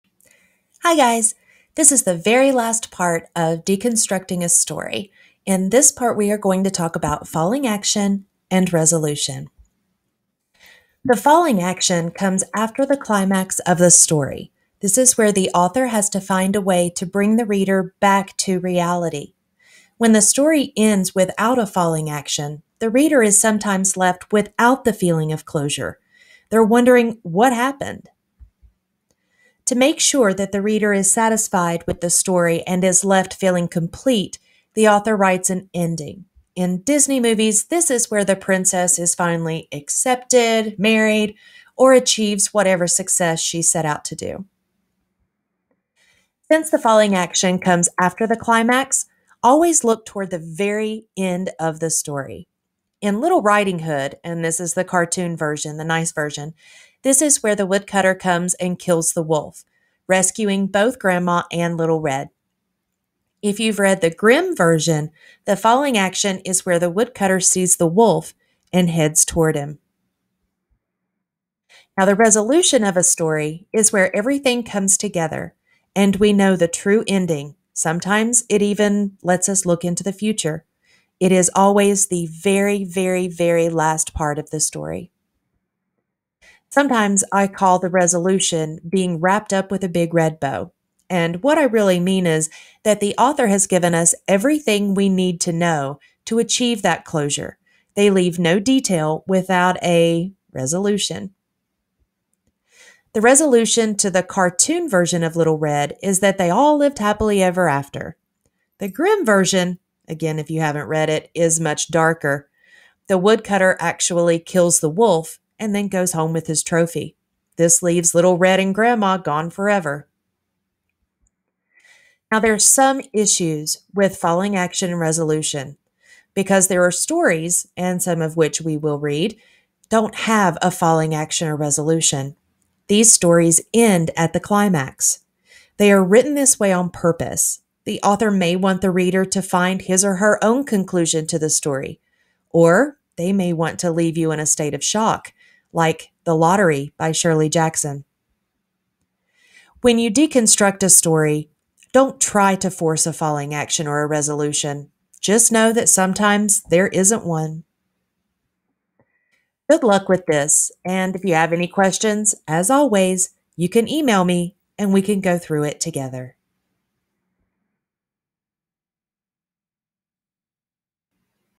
Middle School English: Deconstructing a Story: Falling Action and Resolution
This will help middle school students understand the falling action and resolution of a story.